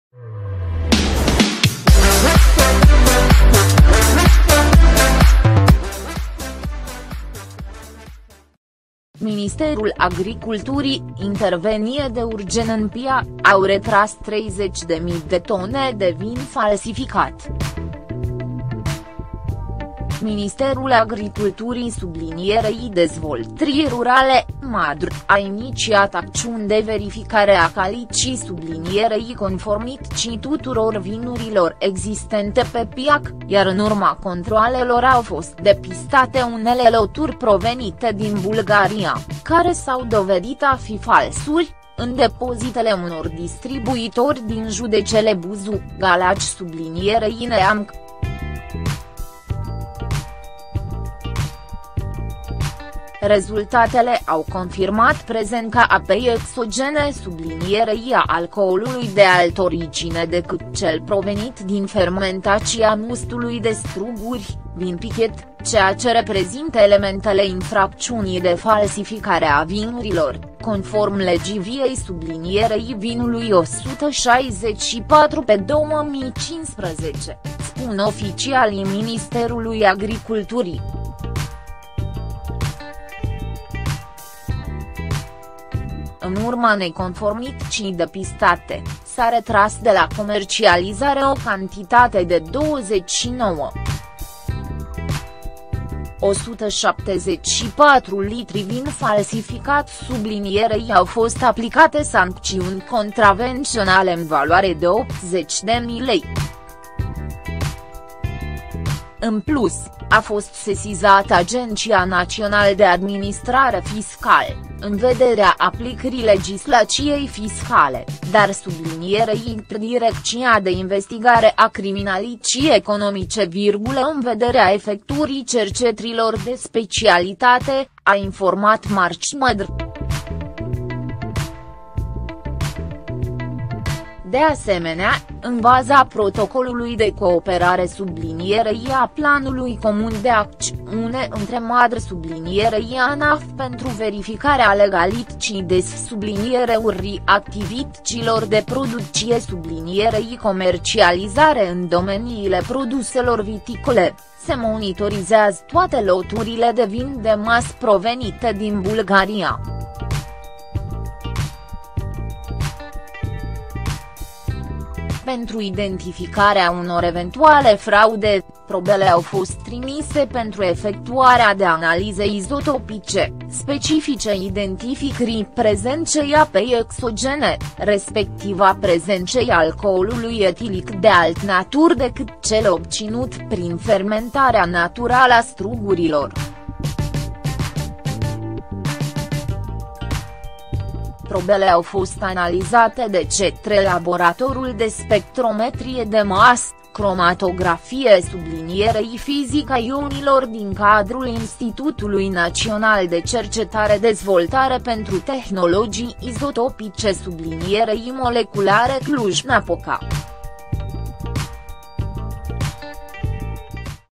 Ministerul Agriculturii, intervenție de URGENȚĂ în piață: au retras 30.000 de tone
Ministerul Agriculturii, intervenție de URGENȚĂ în piață: au retras 30.000 de tone de vin falsificat